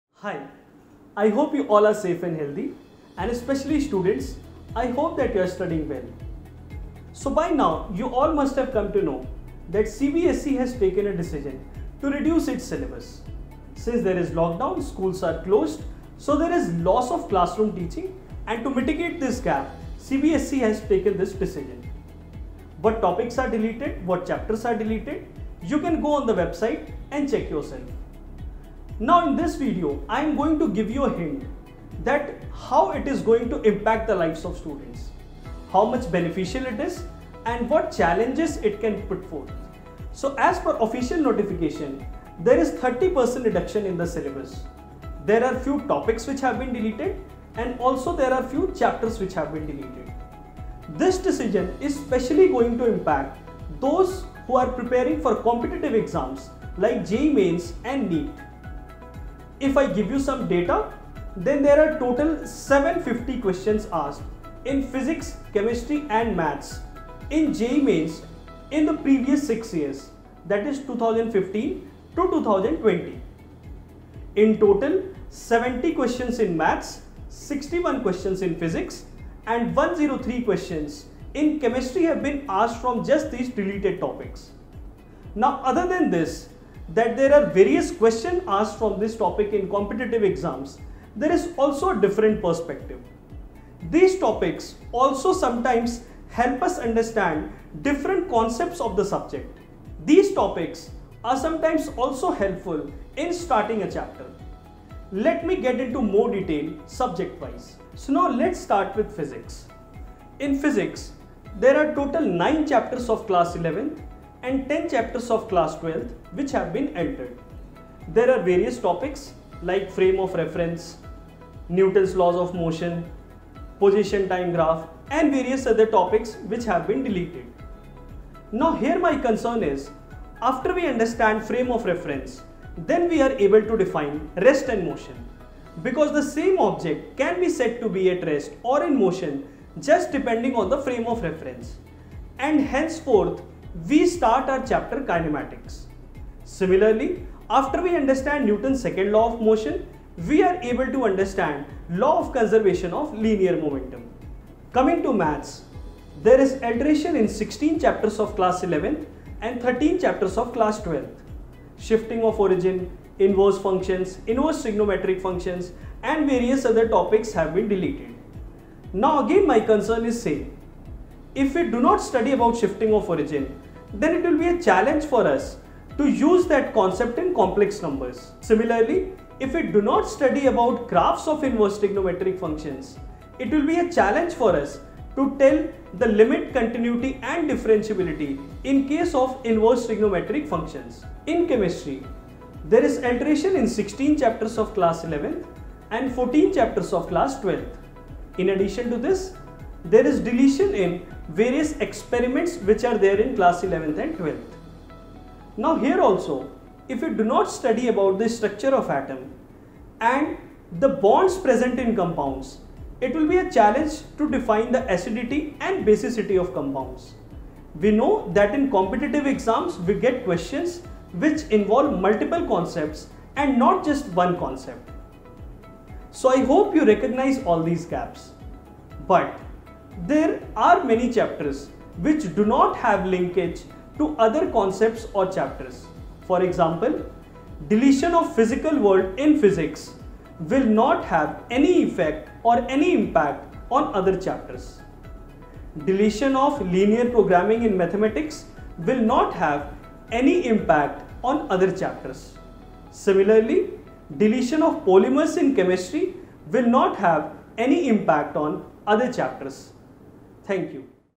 CBSE Syllabus deletion || Impact on JEE Main 2021 || Preparation strategy
Recently, the Central Board of Secondary Education(CBSE) announced a 30% reduction in the syllabus for the academic session of 2020-21, considering the academic loss faced by the student due to the nationwide closure of schools in the face of COVID-19 pandemic. This does reduce the academic burden of students in the lower grade but leaves the student of 10+2 in a state of confusion, as these students prepare for different competitive exams along with their regular curriculum.
JEE Main is one of the most sought after entrance examinations among the medical aspirants, whose syllabus is based on the class XI-XII CBSE curriculum. Here we try to give a detailed analysis of the effect of deleted portions on JEE Main 2021.
If we analyze this decision critically then this alteration seems a bit superficial as to study and understand other chapters well students have to any which ways study many of these deleted topics because of their dependency on other parts of the syllabus. Also, since these topics will not be taught at the schools, so students have to seek out other mediums to study the deleted portions.